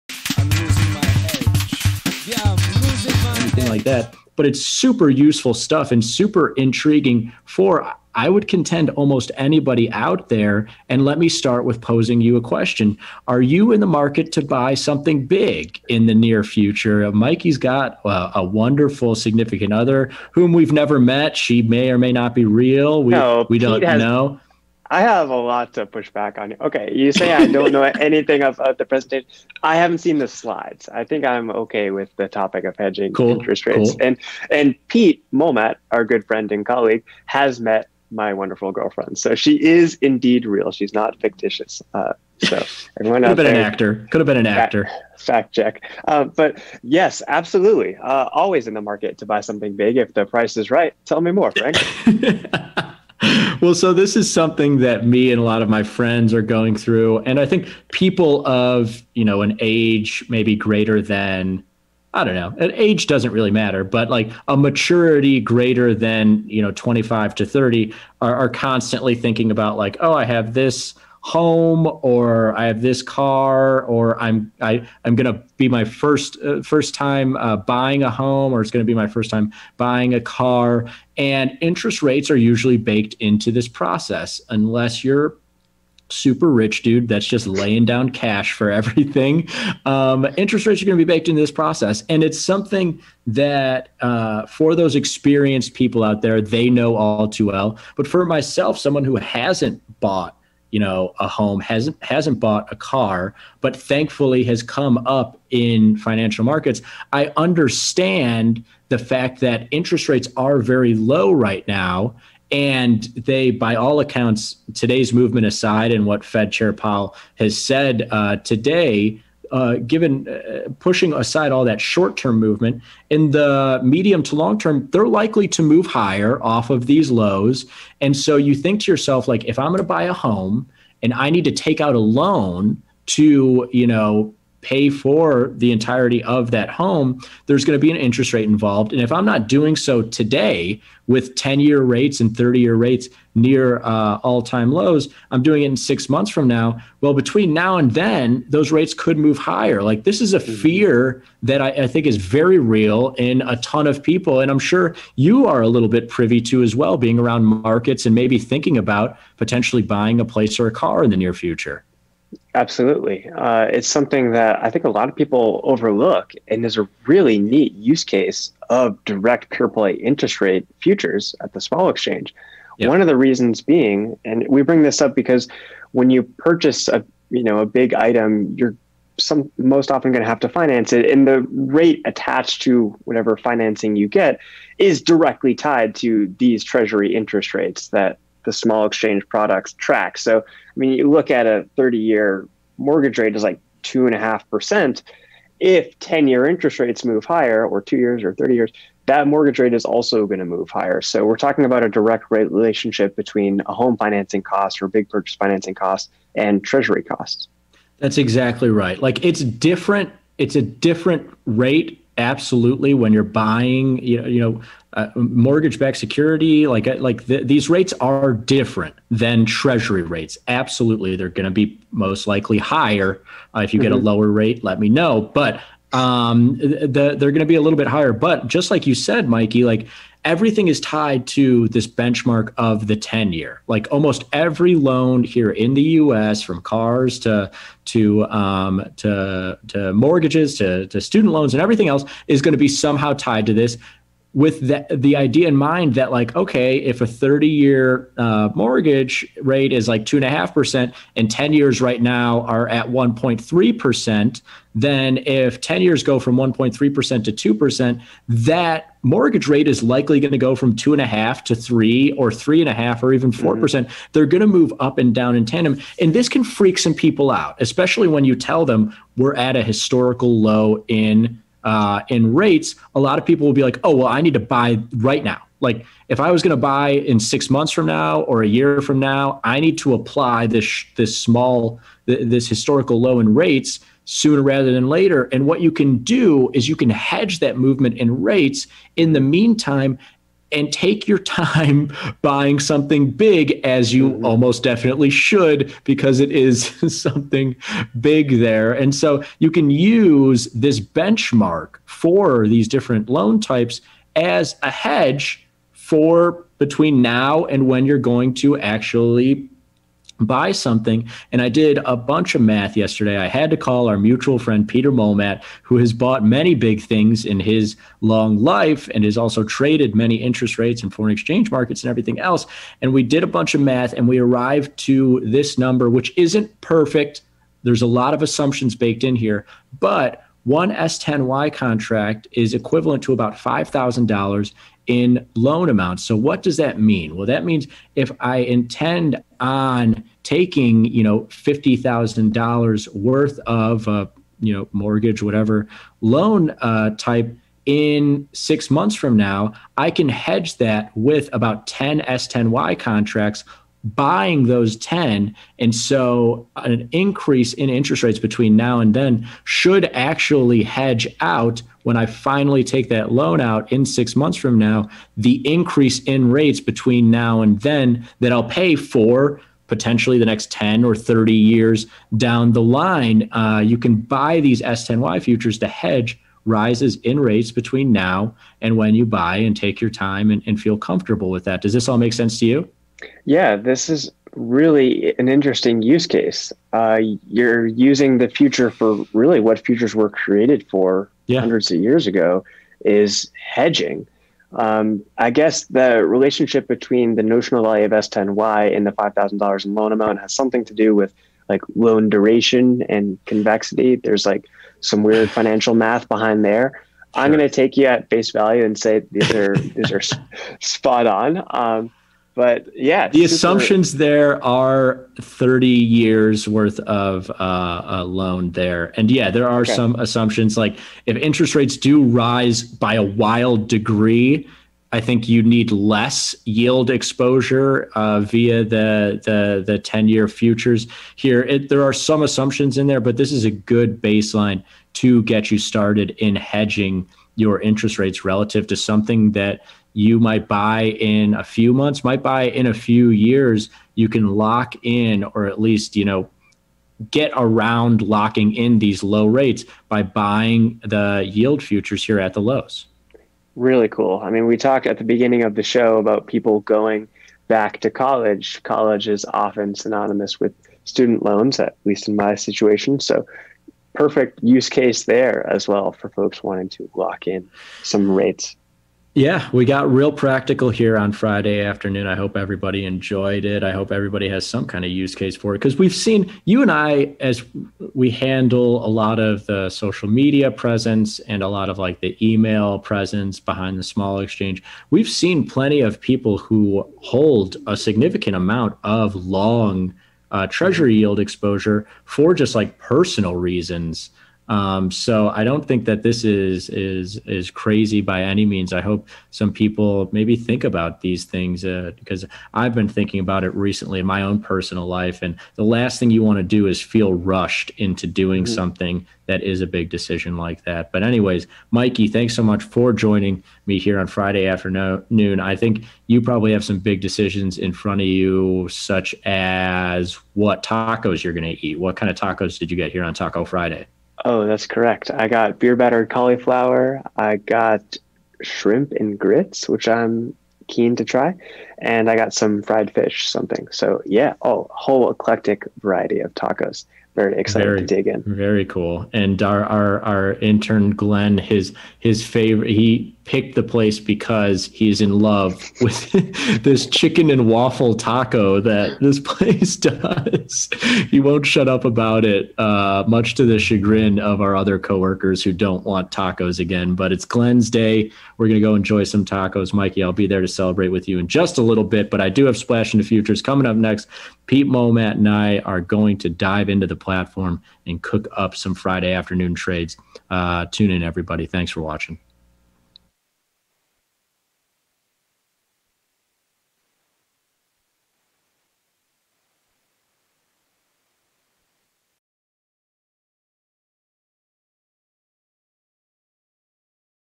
How to Hedge Interest Rates
Worried about interest rates rising before your next big purchase? Frank and Mikey show you how to hedge rate exposure. The guys dive into the vast world of Treasury Yields to show you how interest rates could move in the future, and how you can use futures markets to hedge that movement.